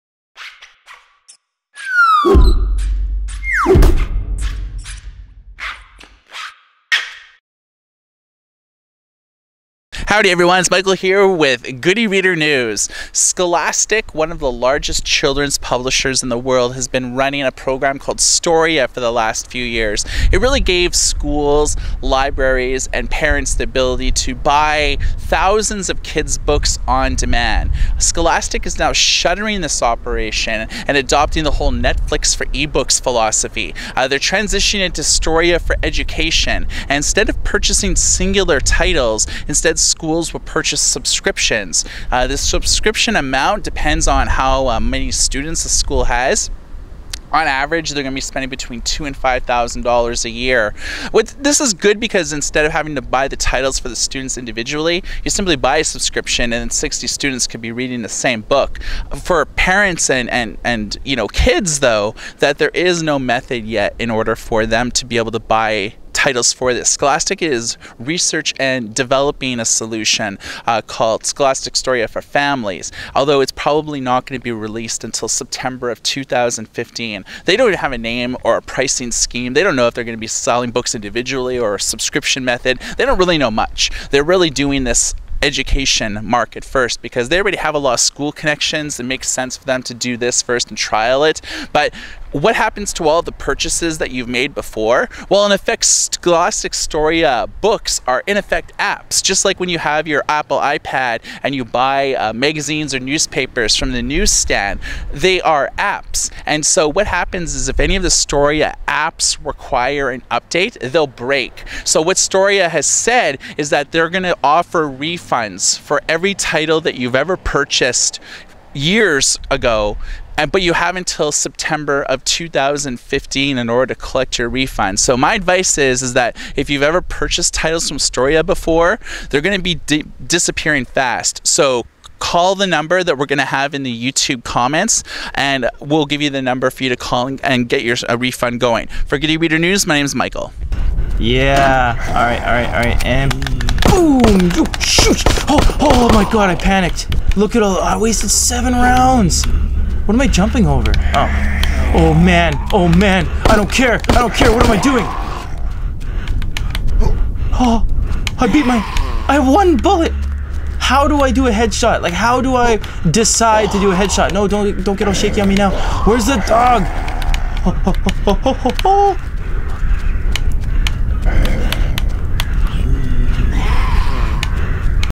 Scholastic Storia is Closing
Scholastic has announced they are stopping selling individual eBooks and embracing the Netflix style subscription model. Books purchased in the past are eligible for a refund at